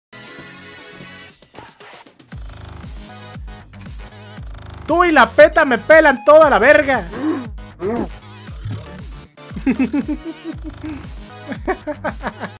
Tu y la peta me la pe**n
Que quede claro que es un juego XD y solo quiero parodiar al meme de Spiderman jajajaja